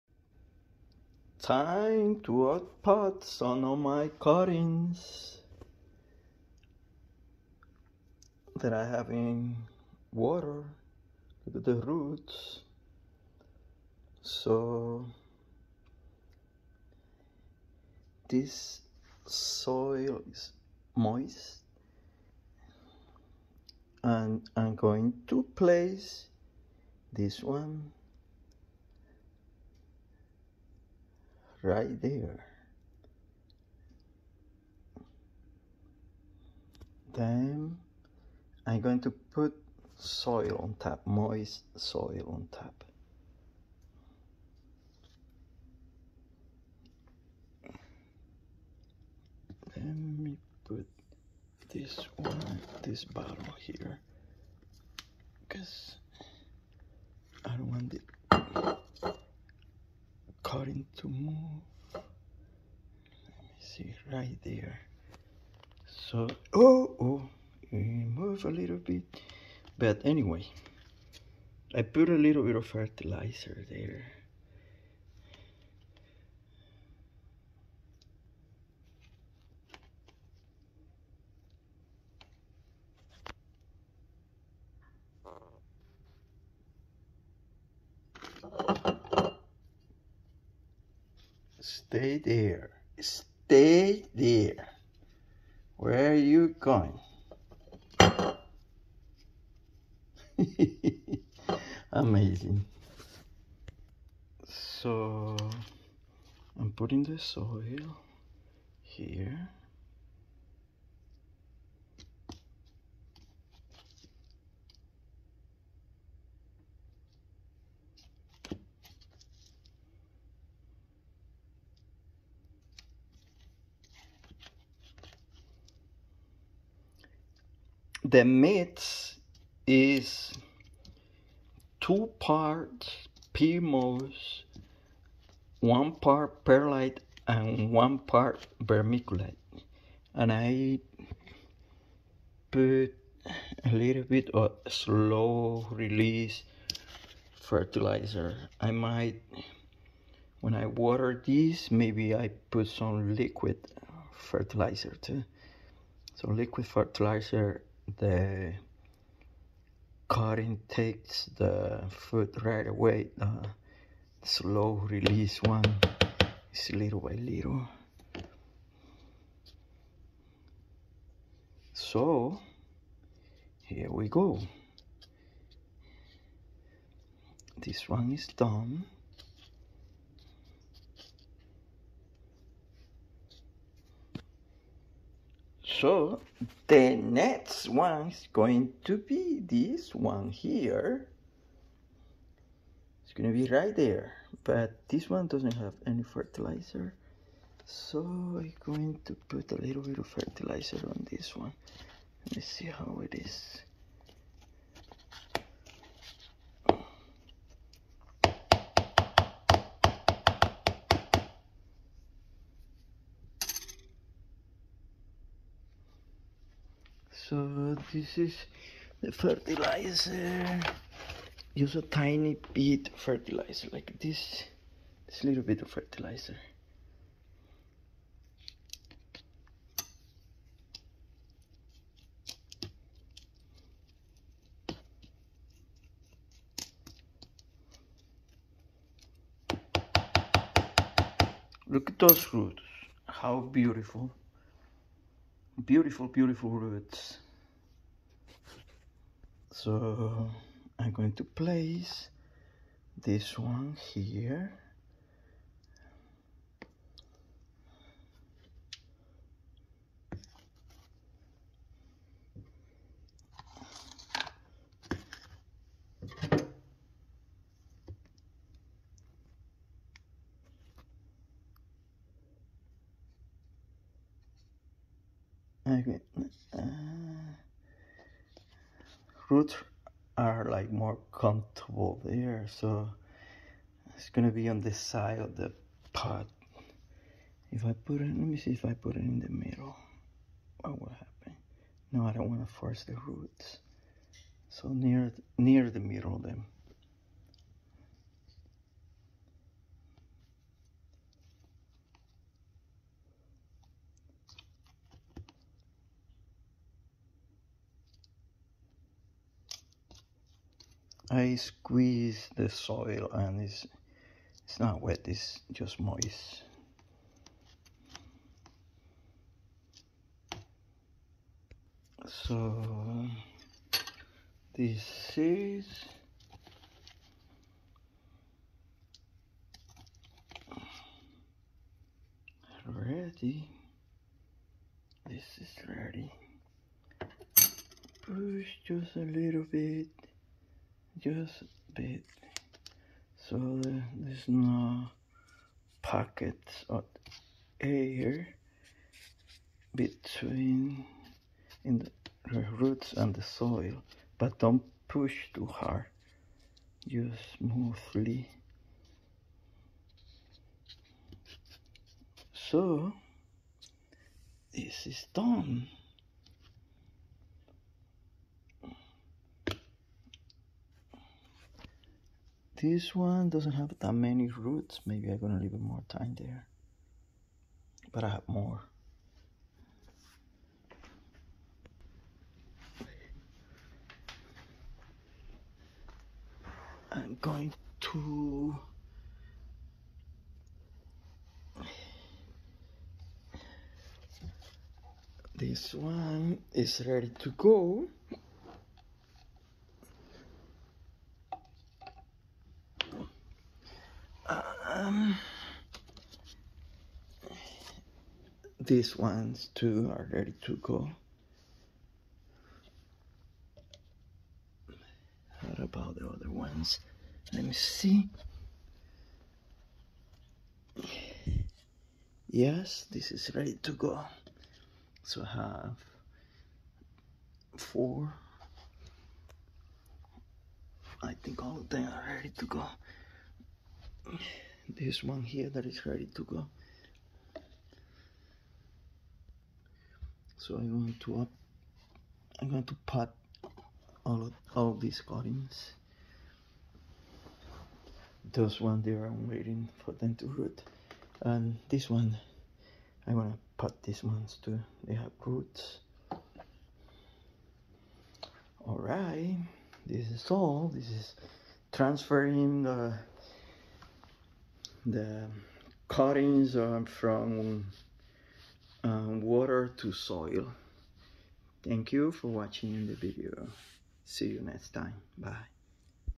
Transferring more cuttings from water to soil!
This cuttings had been  in water for 5 weeks and have plenty of roots!